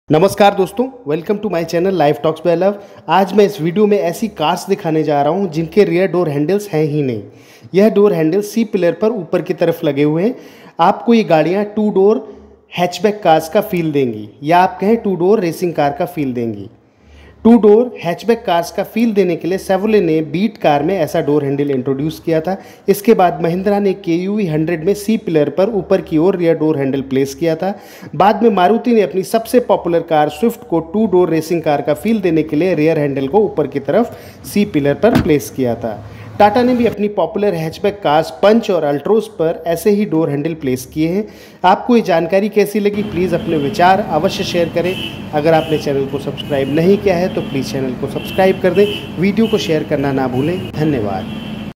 unique stylish door handles ! Amazing Car Door Handles | Hidden Door Handles on C pillar
Indian Car with rear door handles at C pillar